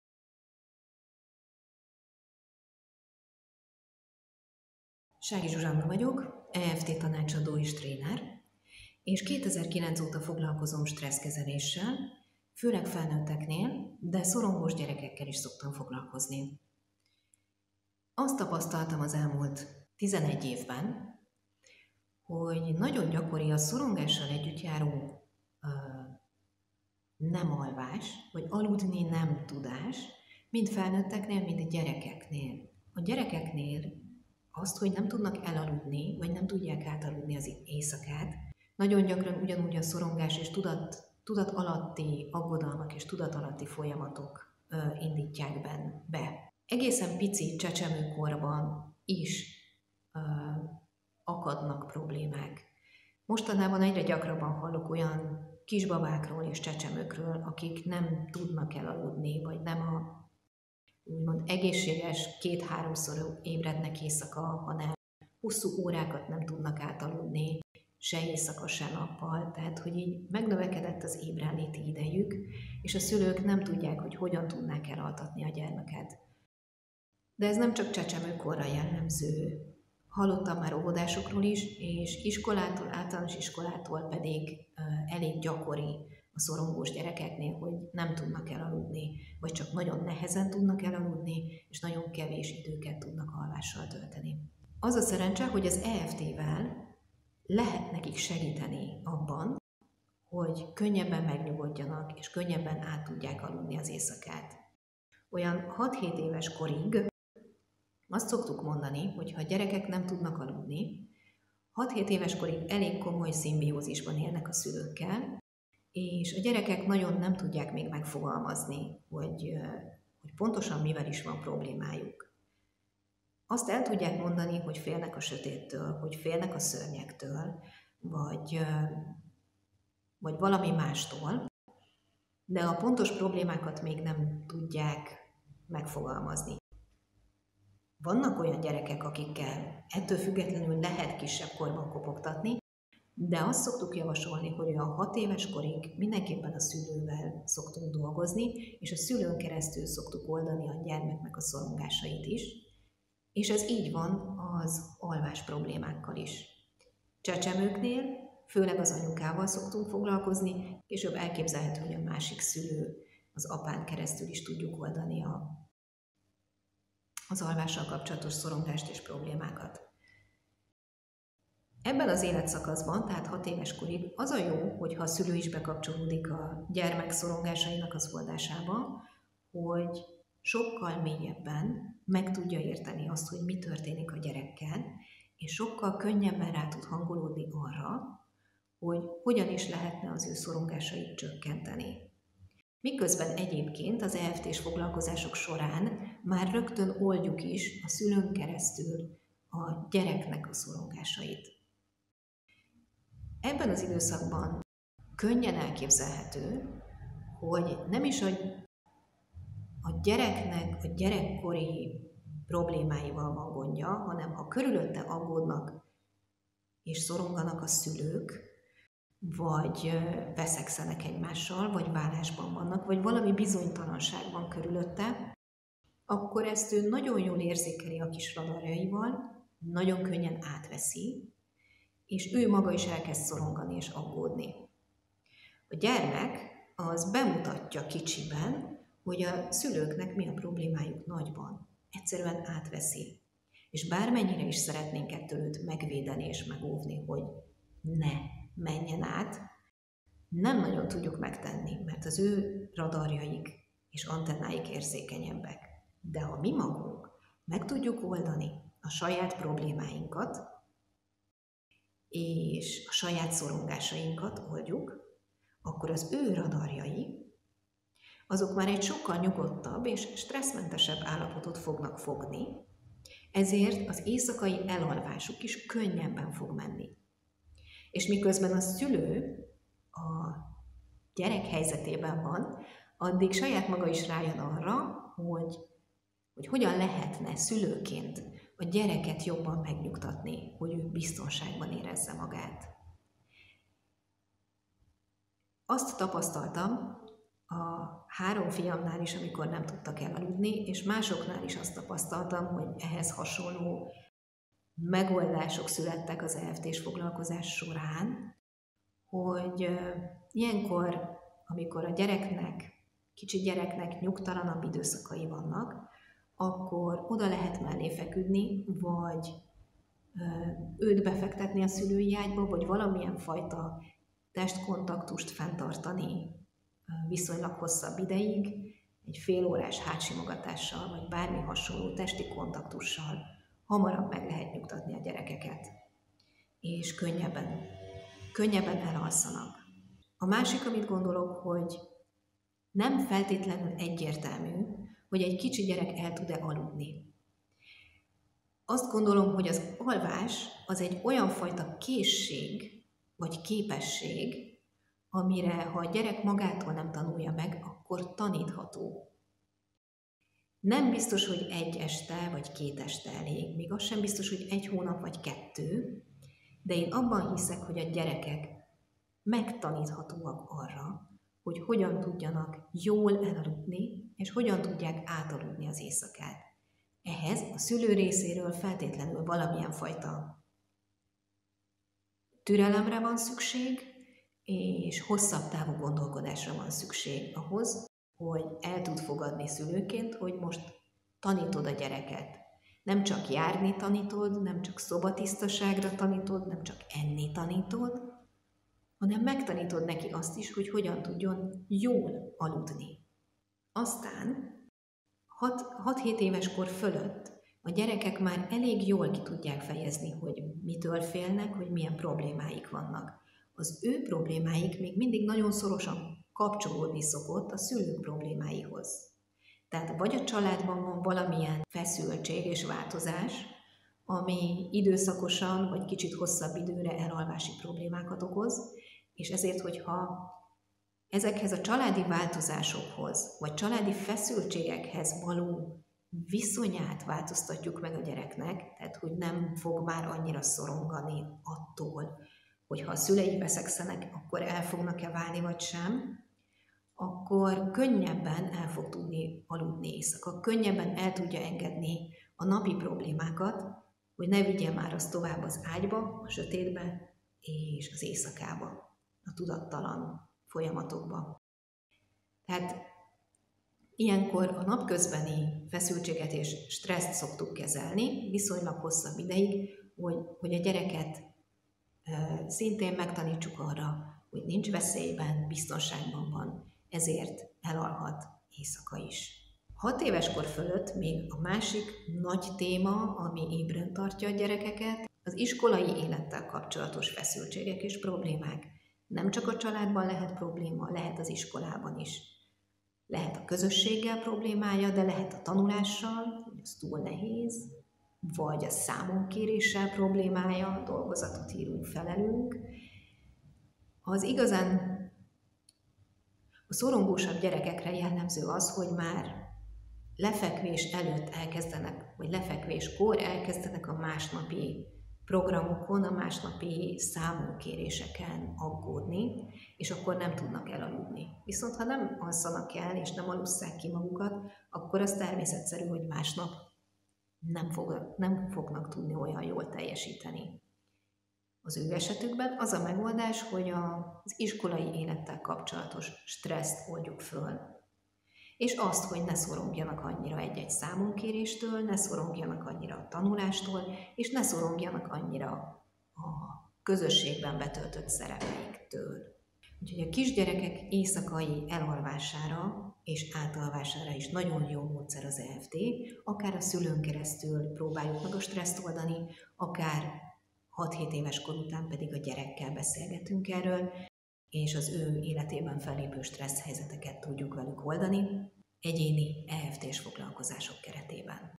Hogyan kezelhető az alváshiány gyerekeknél EFT-vel?
Sok gyerek van különböző életkorokban, aki nem tud elaludni, vagy alváshiánnyal küzd. Ez általában azért van, mert túl sokat szoronganak, de nem mindig tudják megfogalmazni, hogy pontosan mi is a bajuk.
A film arról szól, hogy mit tehetünk, ha a mi gyerekünk is alváshiánnyal küzd, vagy szeretnénk tenni valamit az alvászavara ellen.

Ha úgy érzed, a Te gyerekednek is segíthetne az EFT az alvászavar megszüntetésében, akkor itt találsz meg minket

00:00 Bevezetés
00:20 Problémafelvetés
01:35 Hogyan lehet alvászavarra dolgozni kisgyerekeknél?
03:31 Szülő és a gyerekek problémáinak összefüggése
05:01 Saját tapasztalatok
05:48 Alvásra tanítani a gyereket
06:48 Nagyobb gyerekek problémái családban
08:11 Nagyobb gyerekek iskolai problémái
09:10 Összefoglalás